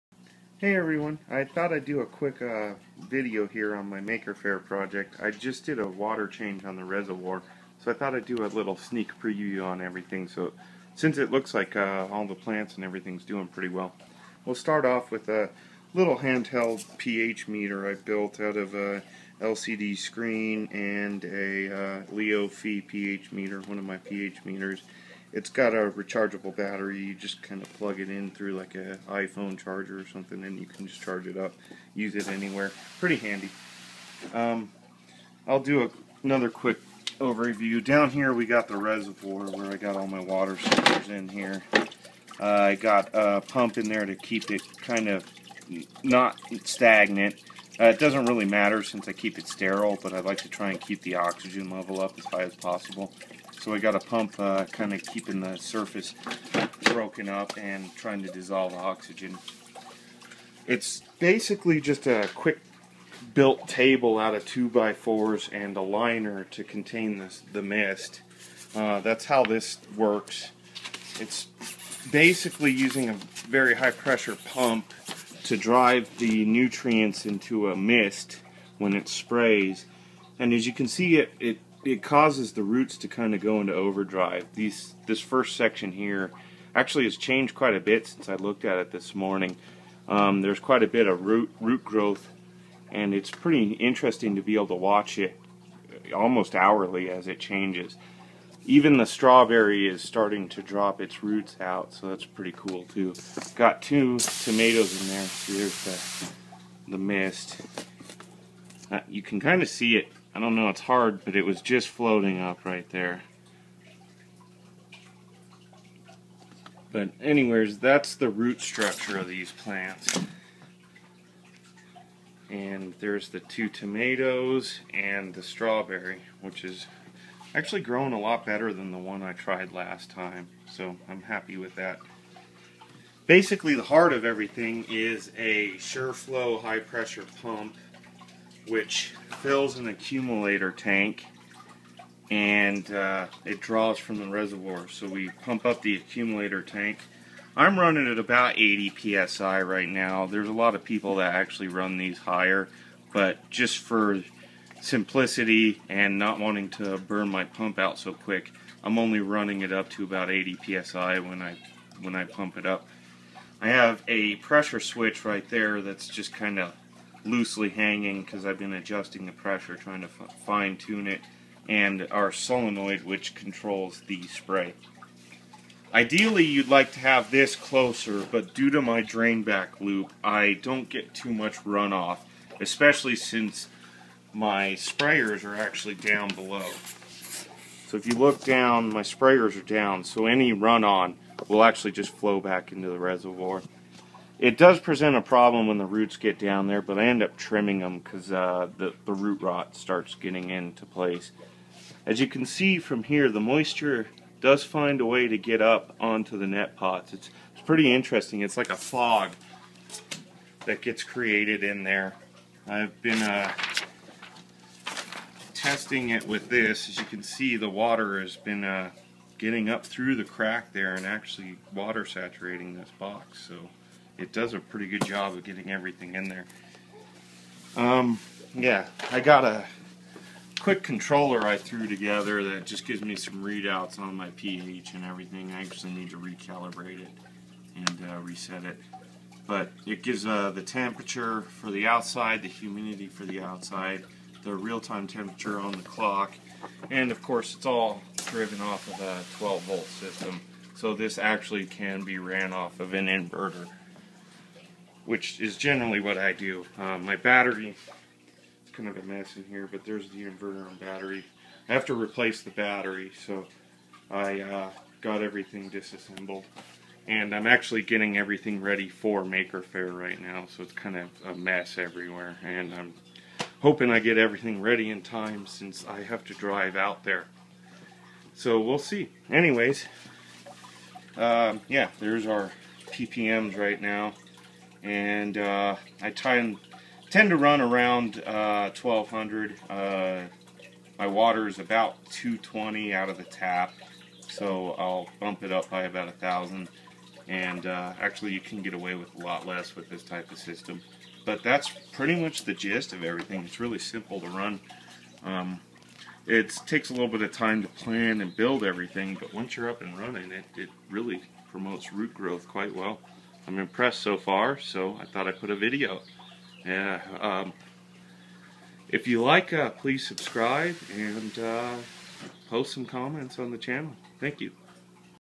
Aeroponics Table Preview
A brief video previewing my Aeroponics table. This was my makerfaire2013 project